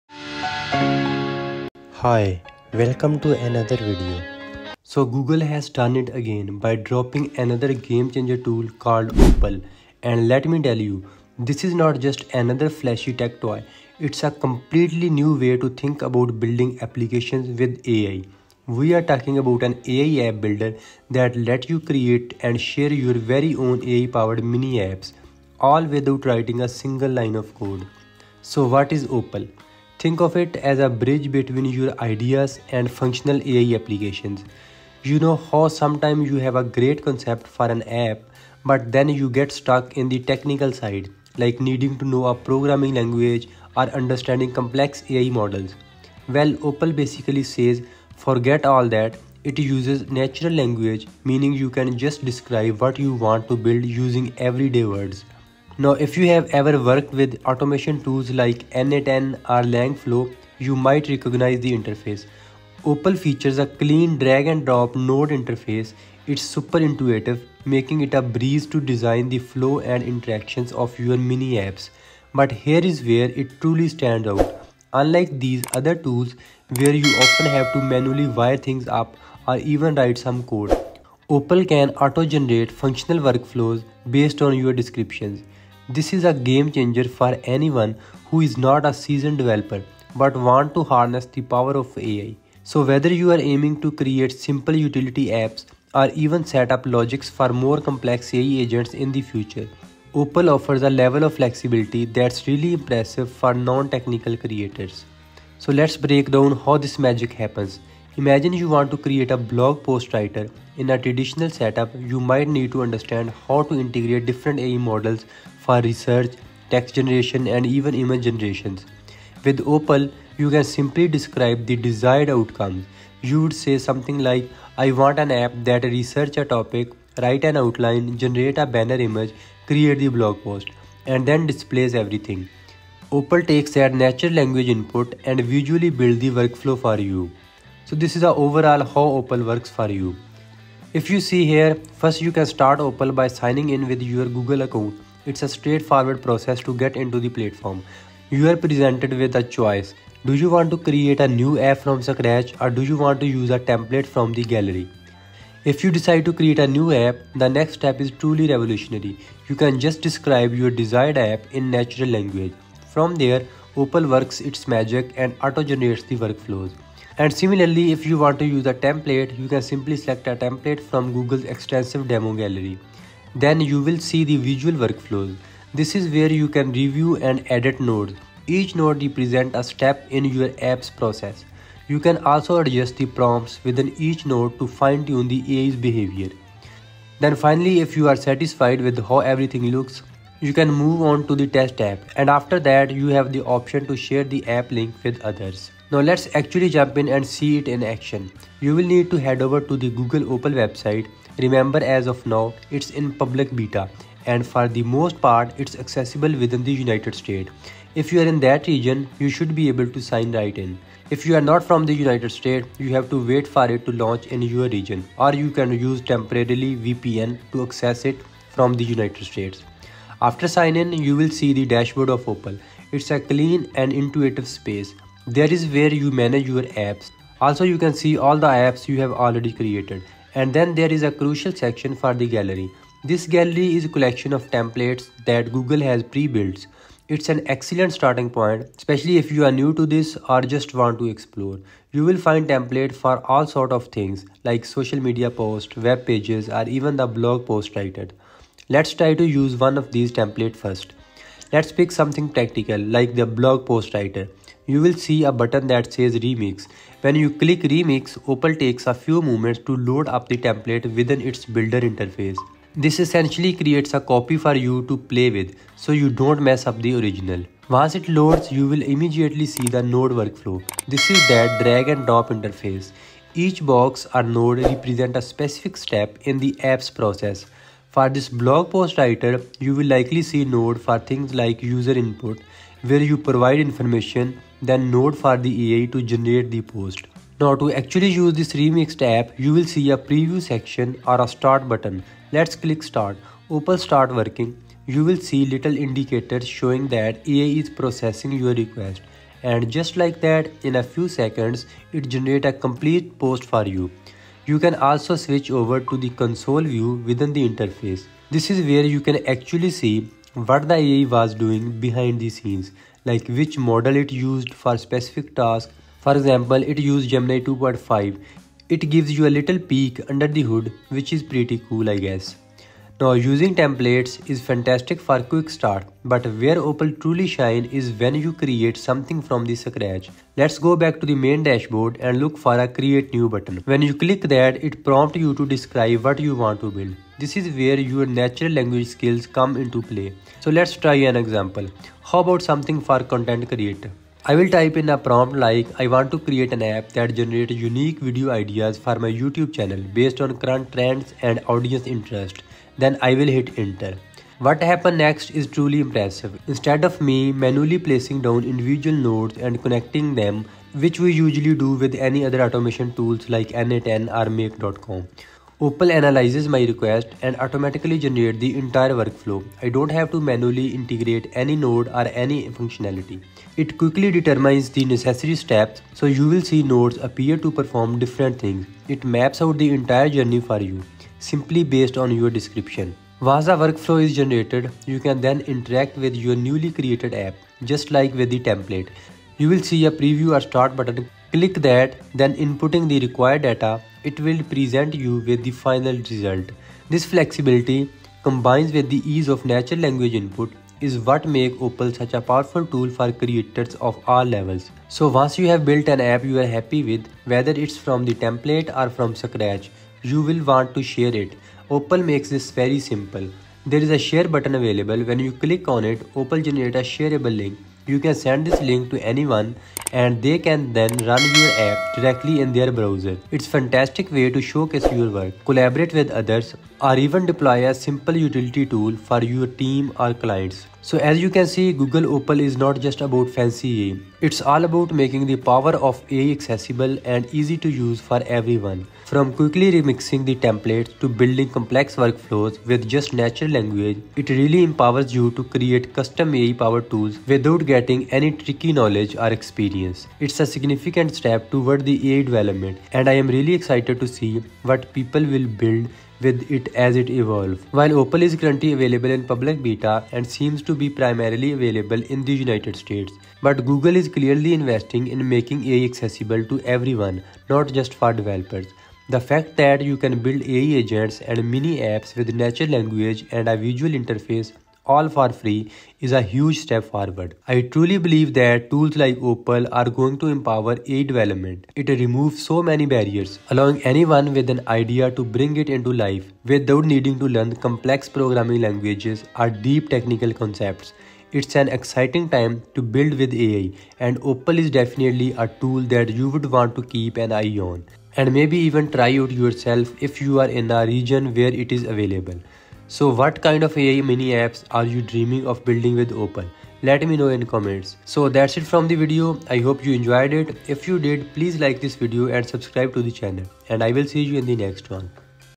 Google Opal App Builder is FREE! Build AI-Powered Apps with Gemini 2.5 & Veo 3 Access
Google just dropped Opal, a game-changing free AI app builder that lets anyone create full web apps using natural language — no coding needed. In this video, I walk you through everything you need to know: from using Gemini 2.5 Pro, integrating Veo 3, to building your first app.

Whether you're a developer, entrepreneur, or complete beginner — this tool might replace no-code platforms like Bubble or Glide for good. Watch till the end to see real-time app creation in action!